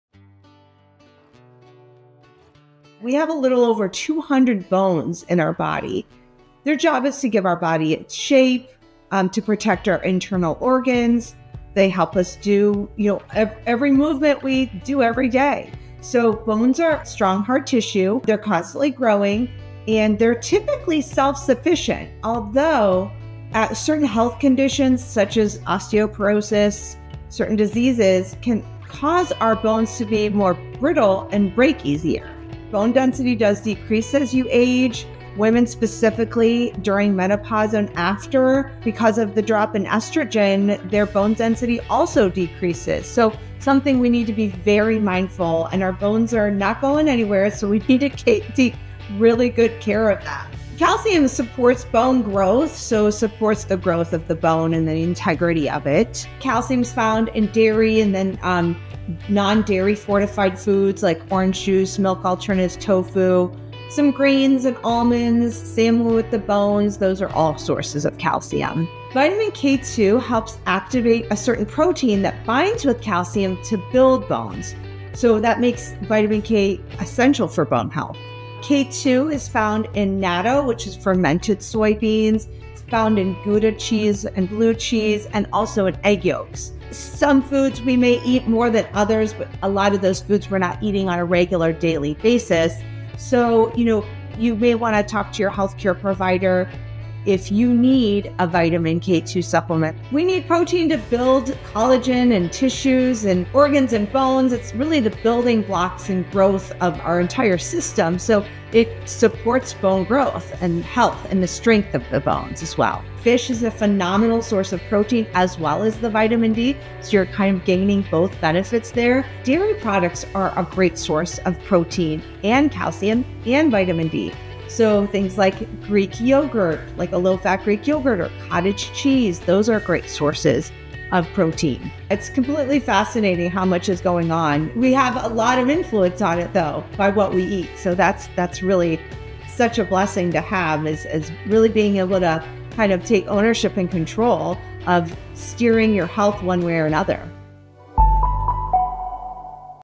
Improving Bone Health Through Diet
Osteoporosis silently weakens your bones, which can make you more likely to experience a bone fracture (broken bone). In this video, Julia Zumpano, RD, shares how you can use your diet to help with osteoporosis from worsening or prevent it altogether.

Chapters:
0:00 What are bones?
0:35 Does bone density decrease as you age?
0:53 Why calcium is important
1:10 Vitamin K2 helps with bone health
1:40 Protein is essential for bone strength
2:00 Conclusion